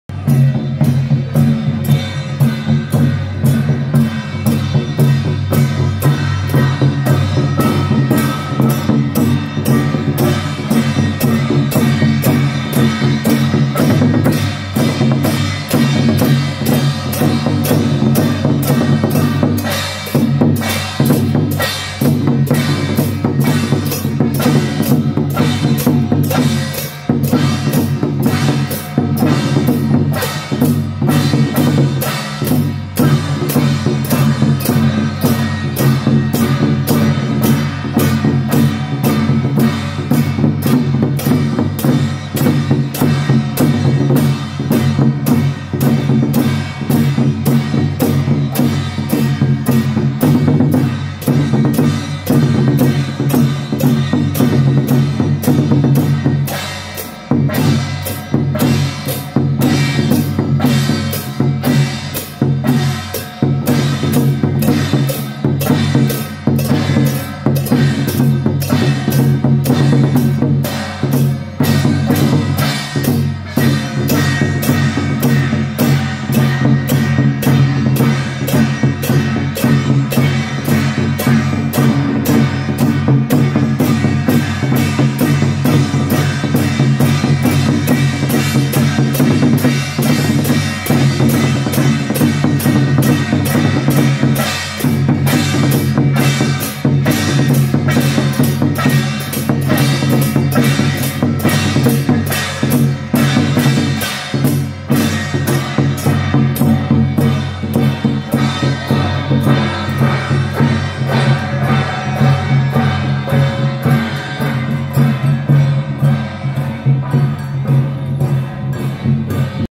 【台中青山會-麻豆鼓】~111臺中拱生堂地藏王菩薩北巡新莊地藏庵謁祖參香
台中青山會-麻豆鼓